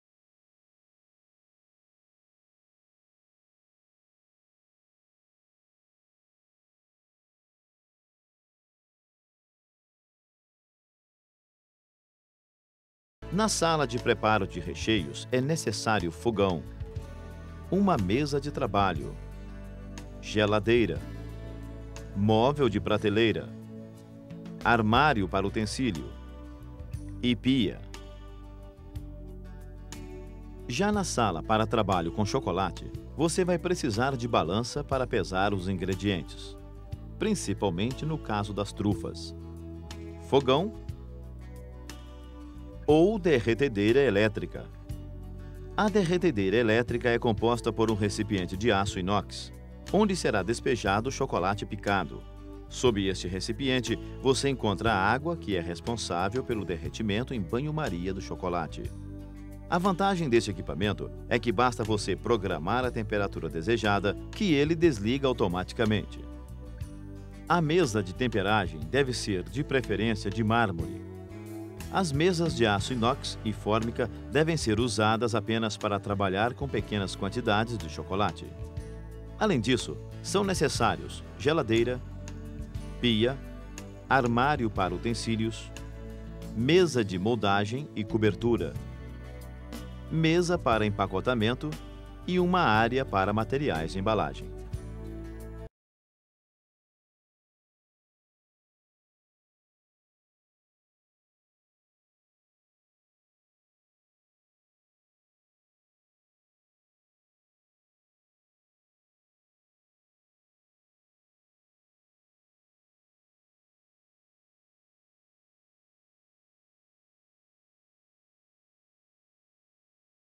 Como Montar e Operar uma Pequena Fábrica de Chocolates - Equipamentos - Cursos CPT
O mercado brasileiro de chocolate artesanal possui um grande potencial.

A diversificação é extensa; vai desde a produção de chocolates comercializados em embalagens sofisticadas, para atender a um público de maior poder aquisitivo, até chocolates feitos sob encomenda para lojas e festas.

Este curso fornece informações básicas para a produção de chocolate artesanal.

Este curso foi produzido em convênio do CPT com o Instituto Fibourg.